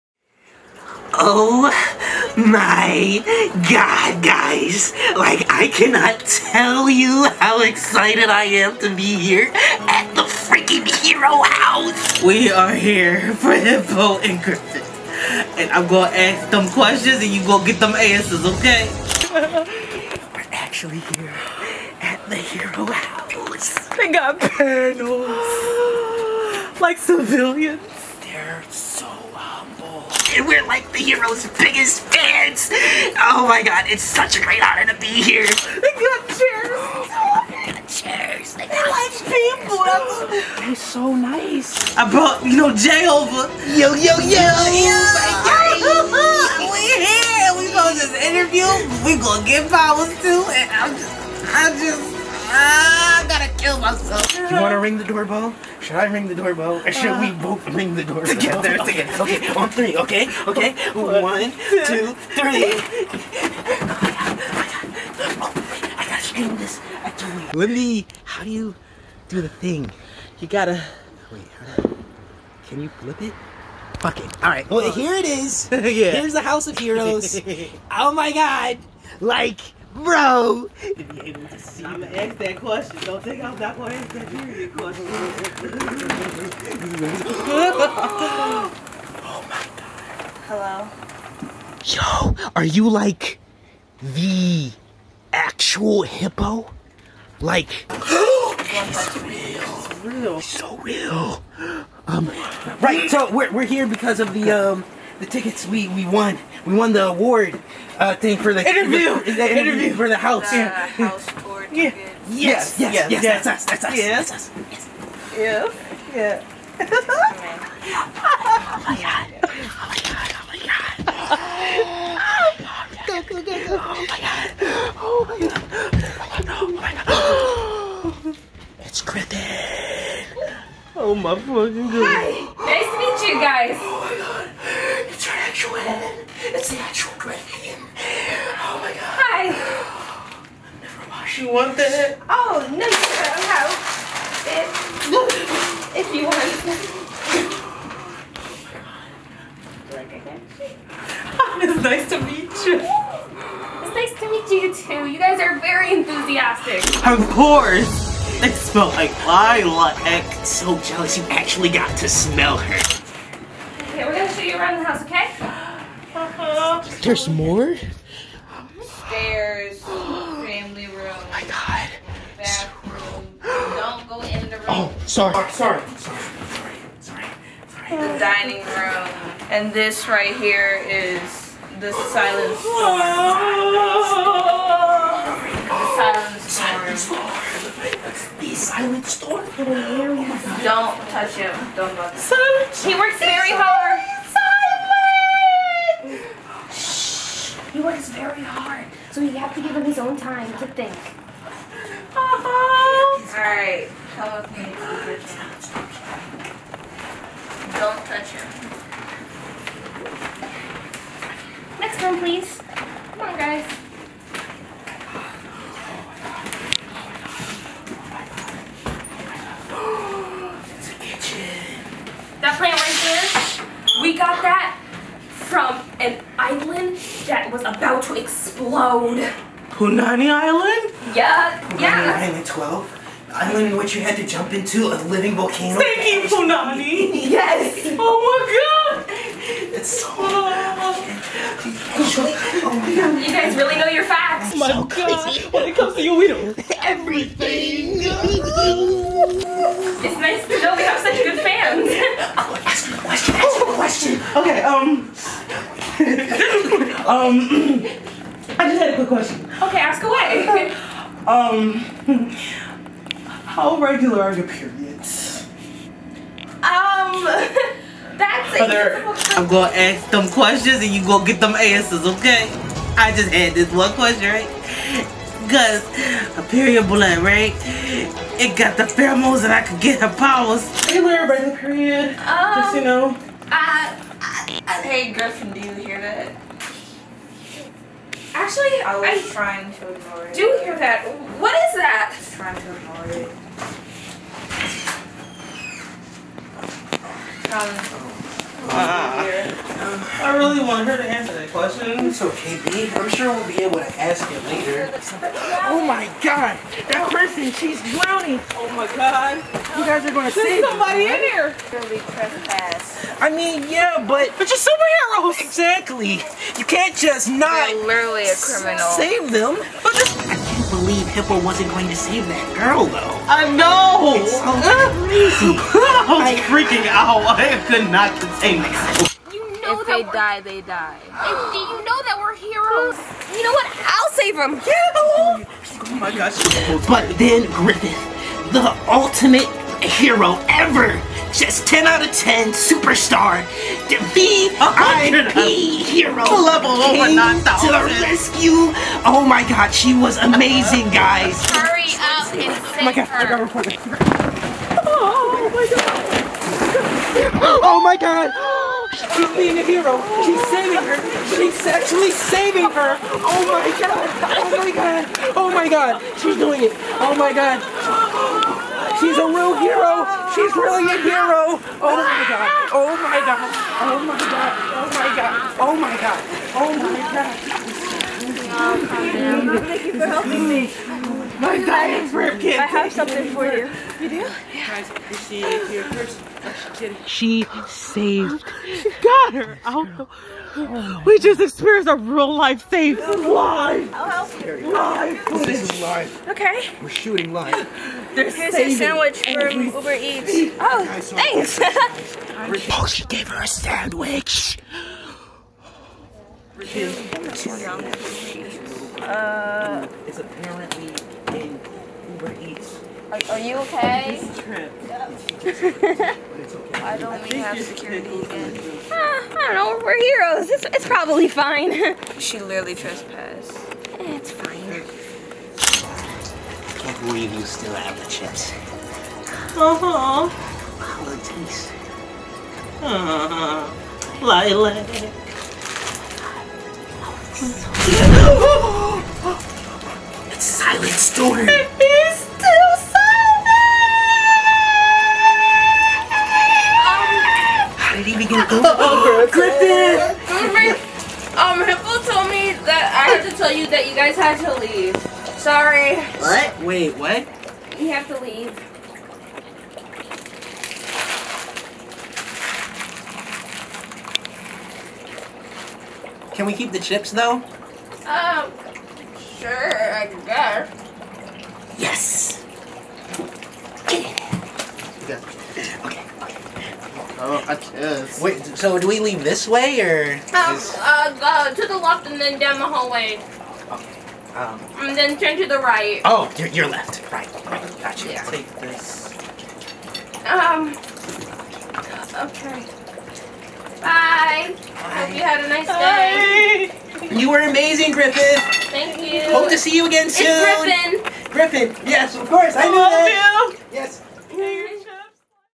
Superhero House
2 superhero fanatics win the tour of a lifetime, I hope u like it its pretty funny.